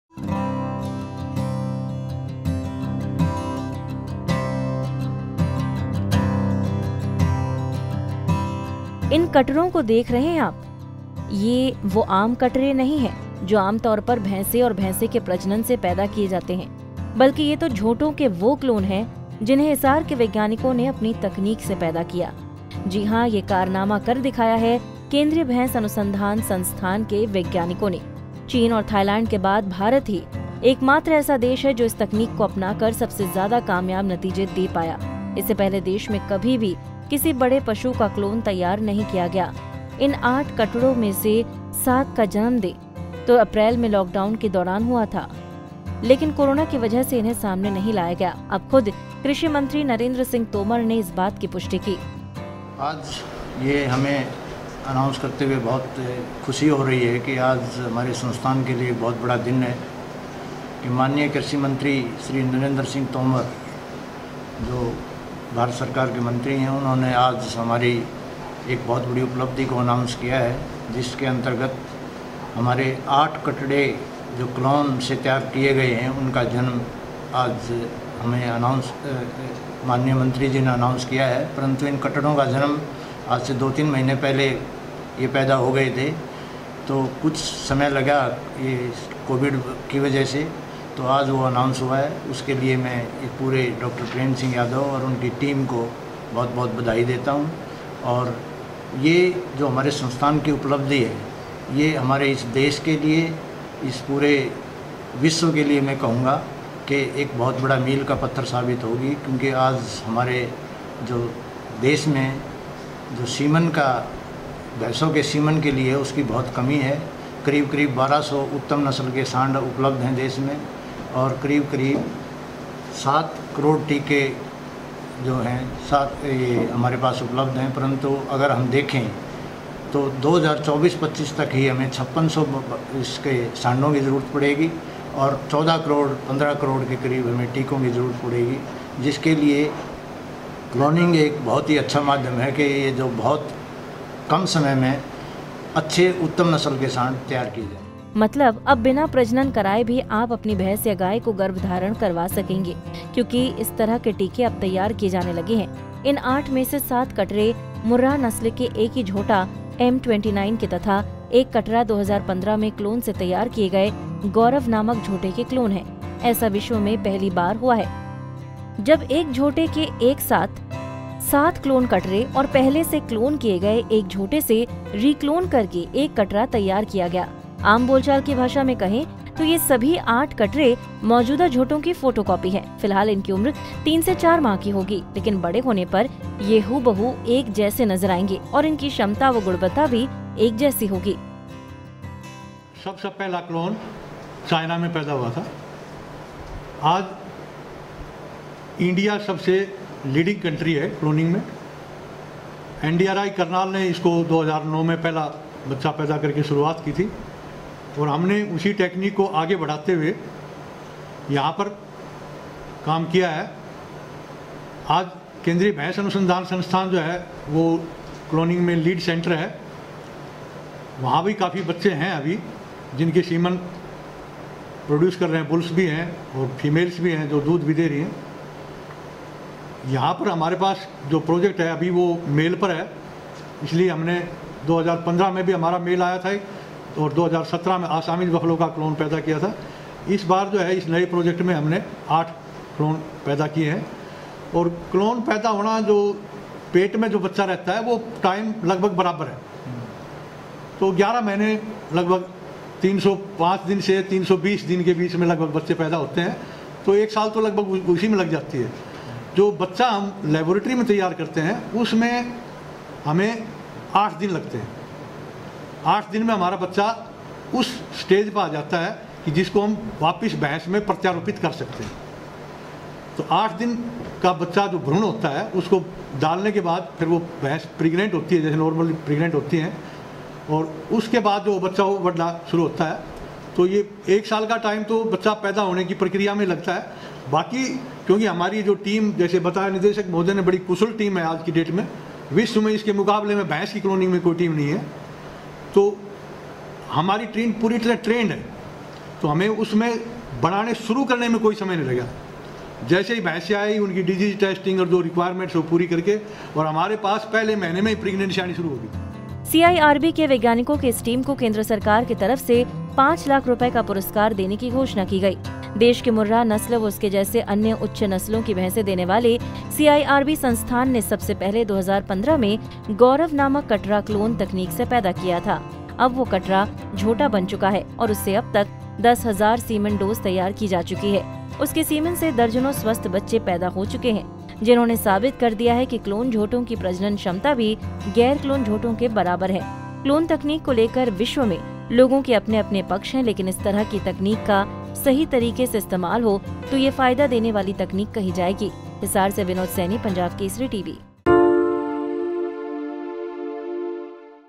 केंद्रीय भैंस अनुसंधान संस्थान के वैज्ञानिकों को मिली बड़ी कामयाबी, कटड़ों के तैयार कर दिए 7 क्लोन
इन कटड़ों को देख रहे हैं आप, ये वो आम कटड़े नहीं हैं, जो आमतौर पर भैंस और भैसे के प्रजनन से पैदा किए जाते हैं, बल्कि ये तो झोटों के वो क्लोन हैं, जिन्हें हिसार के वैज्ञानिकों ने अपनी तकनीक से पैदा किया है, जी हां, ये कारनामा कर दिखाया है केंद्रीय भैंस अनुसंधान संस्थान के वैज्ञानिकों ने, चीन और थाईलैंड के बाद भारत ही एकमात्र ऐसा देश है जो इस तकनीक को अपनाकर सबसे ज्यादा कामयाब नतीजे दे पाया है, इससे पहले देश में कभी भी किसी बड़े पशु का क्लोन तैयार नहीं किया गया है, इन आठ कटड़ों में से साथ का जन्म तो अप्रैल में लॉकडाउन के दौरान हुआ था, लेकिन कोरोना की वजह से इन्हें सामने नहीं लाया गया, अब खुद कृषि मंत्री नरेंद्र सिंह तोमर ने इस बात की पुष्टि की है।